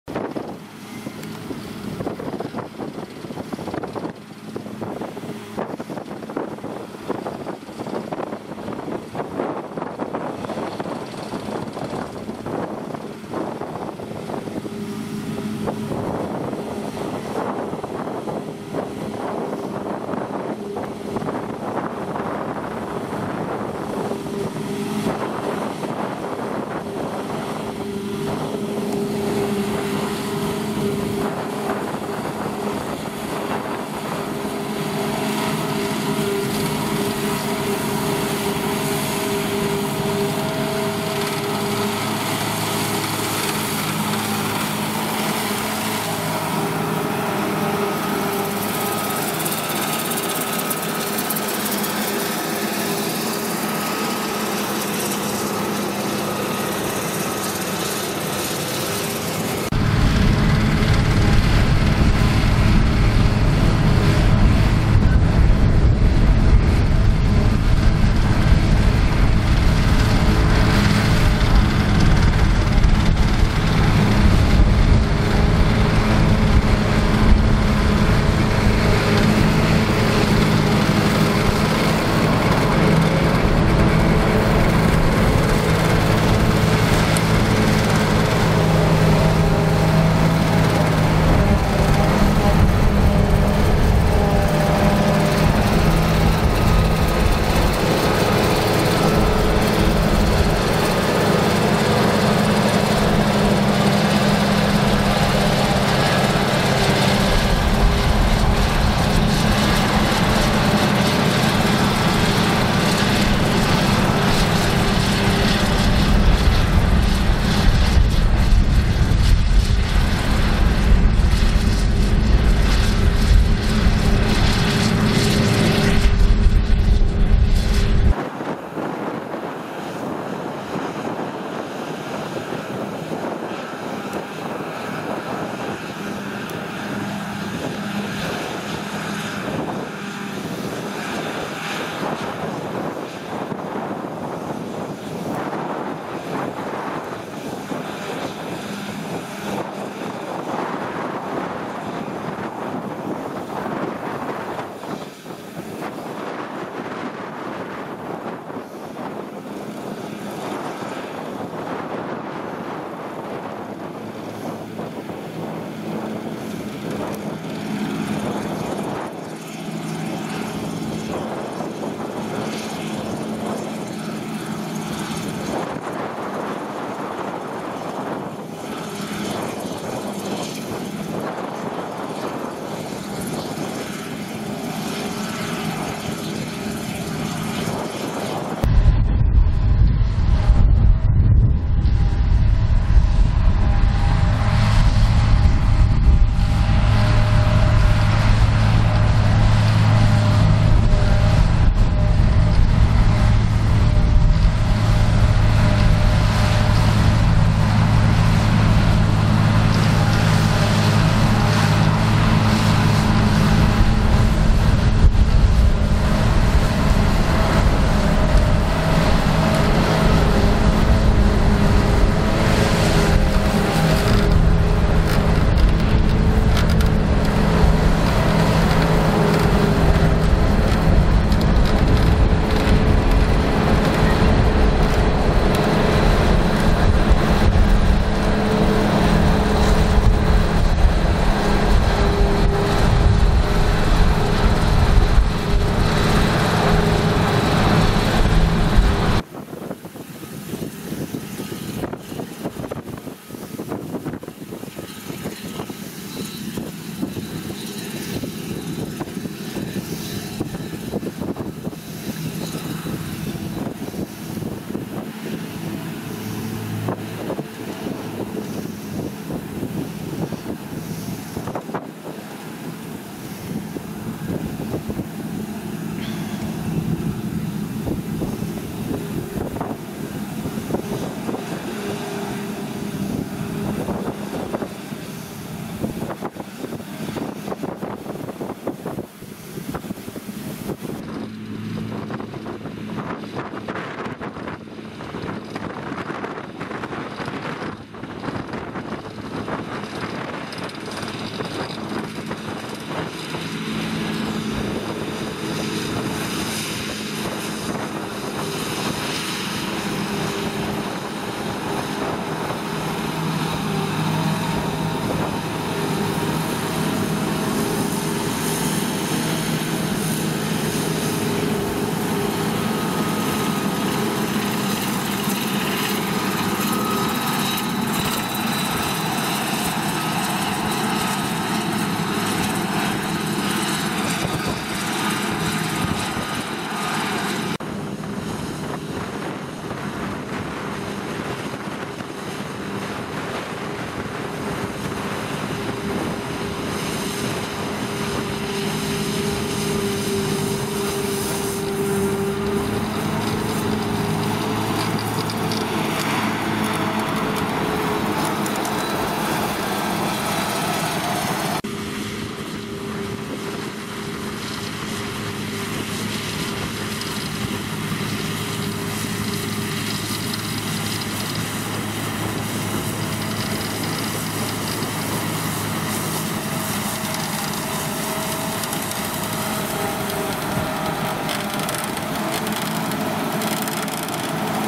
Silage 2020 Daly Agri Wholecrop Silage
Silage 2020 - Team team from Daly Agri lifting a crop of Wholecrop Silage on very windy day near Garryvoe County Cork with a Claas Jaguar 970 Forage Harvester with John Deere & New Holland Tractors each with Broughan Engineering Mega HiSpeed Trailers. A word of thanks to all involved.